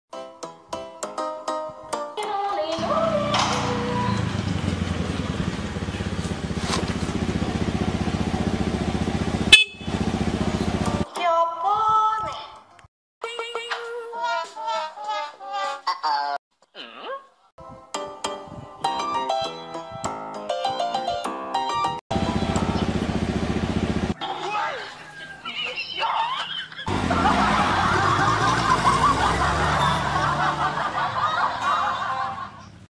Lucu, Akhirnya kabur
Parodi lucu, tak kirain janda tahu tahunya brewokan,Akhirnya kabur, selamat menonton semoga terhibur